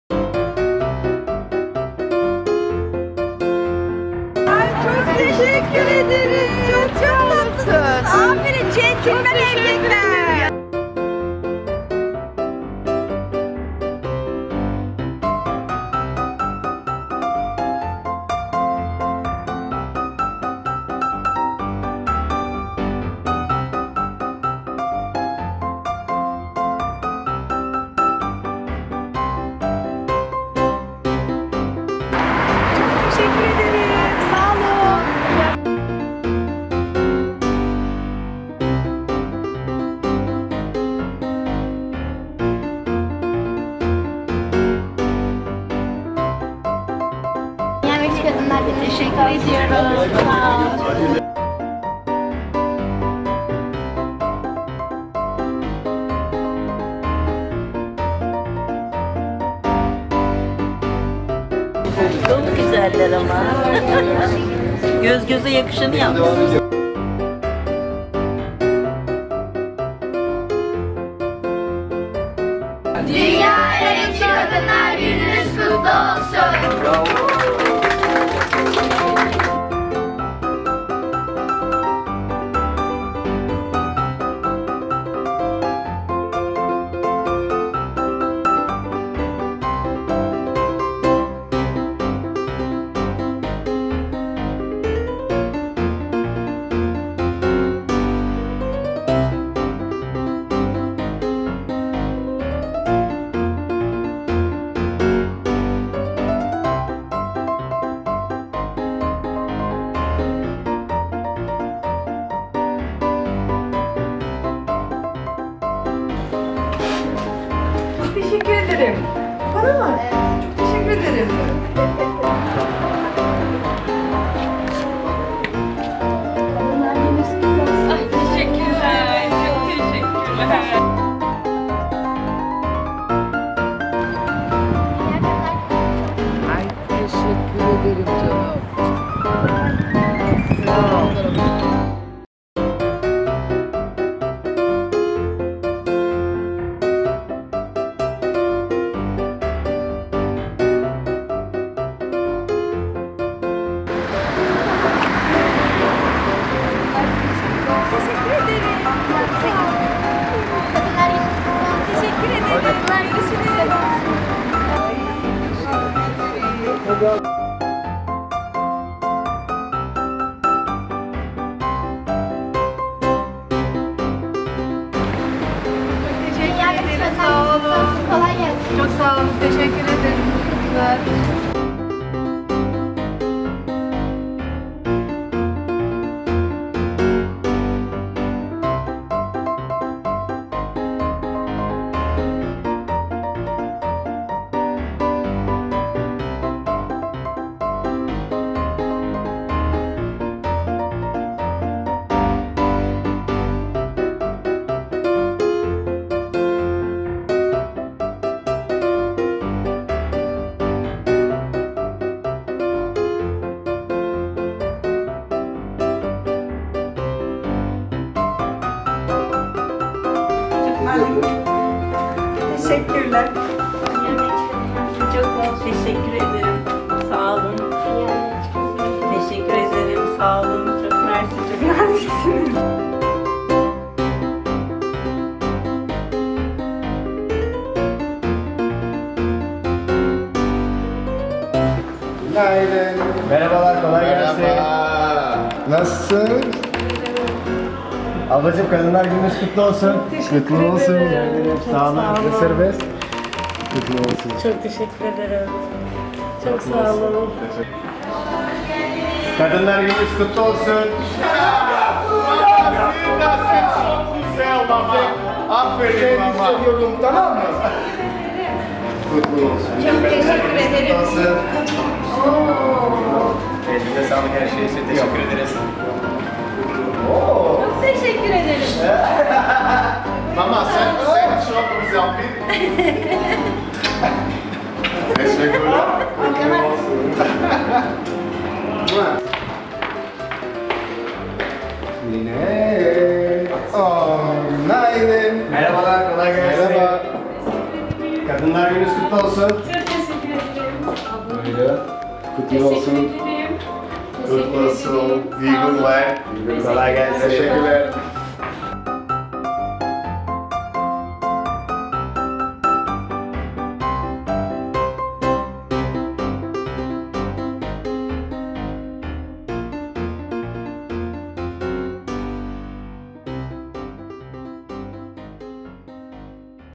Yüzlerinizin Hep Gülmesi Dileğiyle | Kadınlar Gününüz Kutlu Olsun
8 Mart Dünya Emekçi Kadınlar Günü Kutlu Olsun!

Altyapı ve A Takım sporcularımız kulüp çalışanlarımız ve değerli emekçi kadınlarımıza çiçekler vererek KadınlarGünü'nü kutladı.

Sahip olduğumuz tüm güzelliklerin mimarı değerli kadınlarımızın 8 Mart Dünya Emekçi Kadınlar Günü'nü en içten dileklerimizle kutlarız.